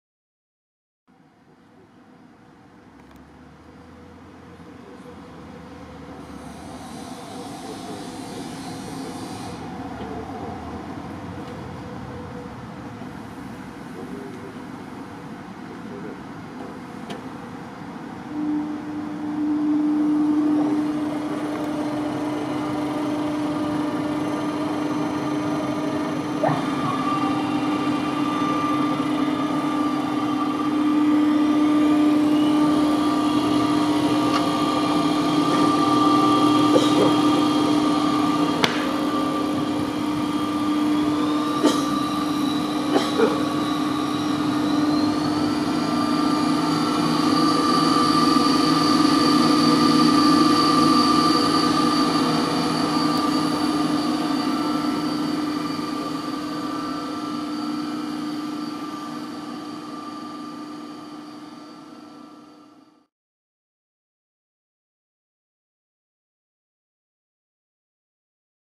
K+G Wetter SM 45 STL Bowl Cutter (Part 1/2)
Short test run of K+G Wetter SM 45 STL, 45 liters bowl chopper with variable knife speed.